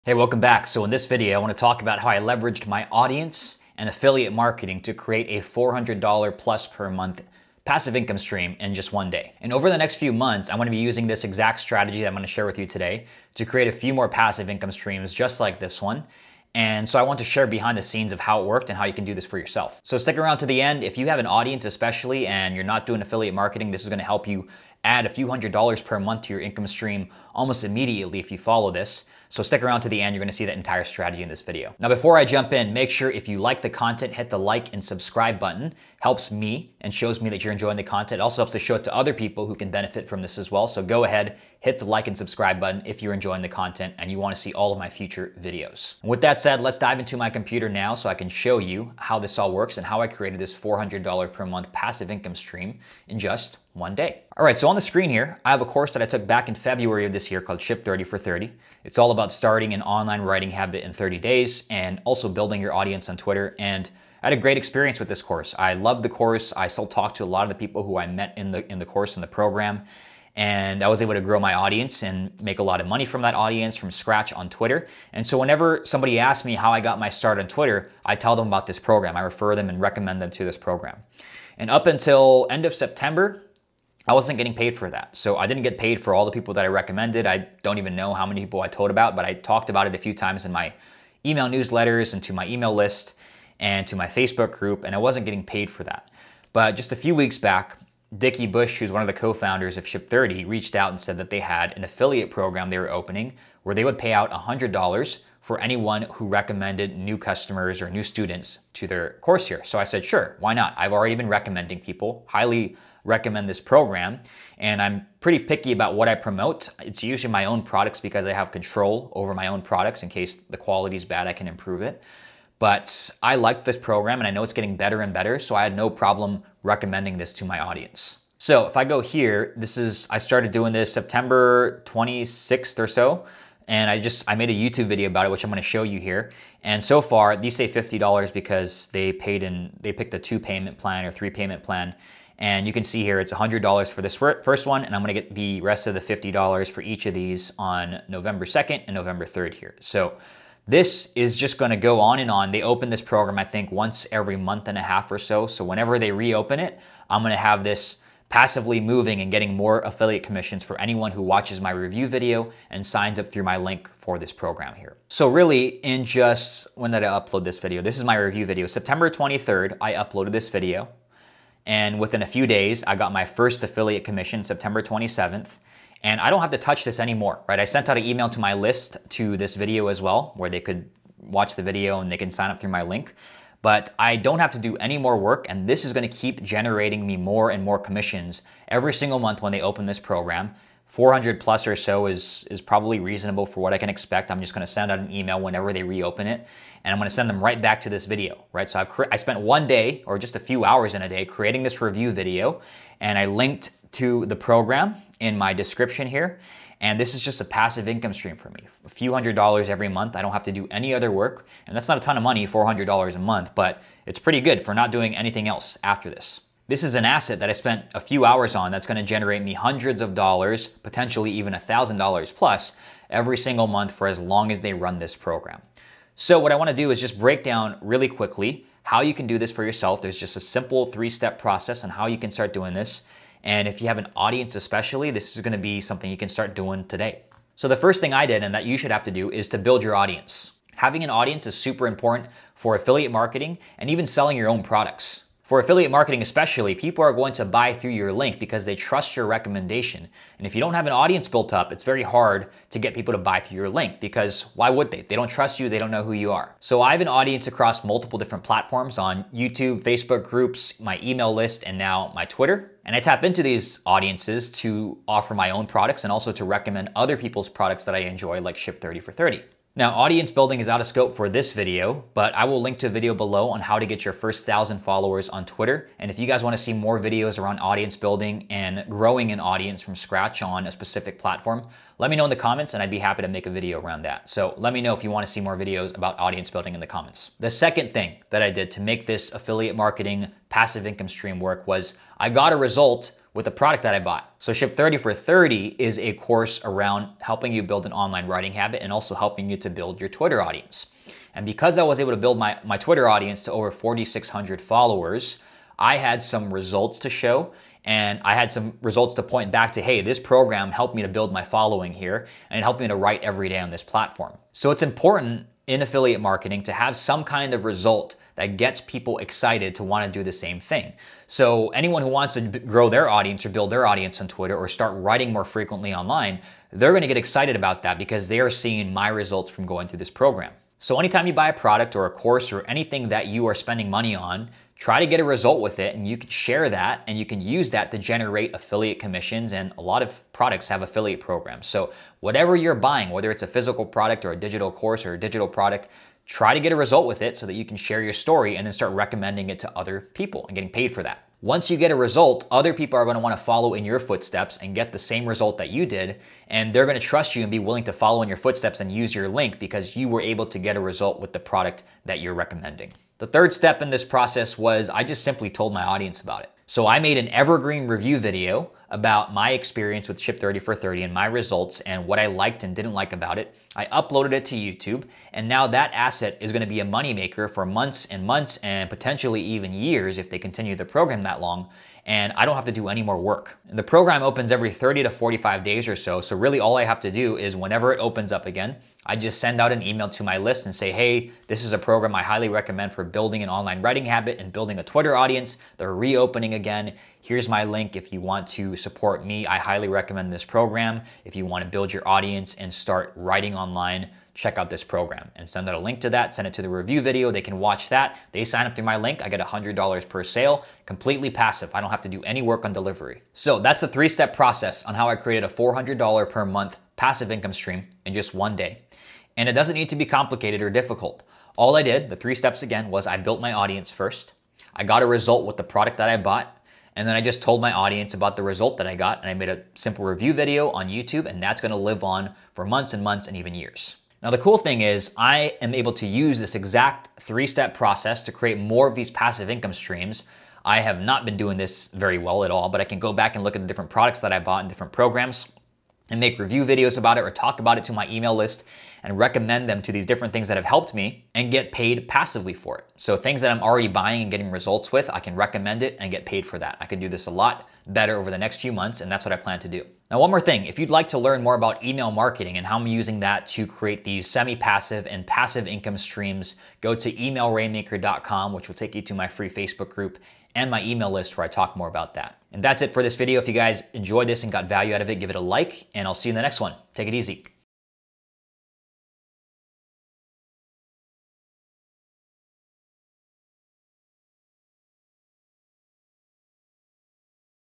Affiliate Marketing: How I Created A $400+ Per Month Passive Income Stream In 1 Day
In this video I reveal how I created a passive income stream through affiliate marketing that will bring in $400 per month.

► Get my "7-Day Superfast Sales Email Challenge" to learn to write emails that sell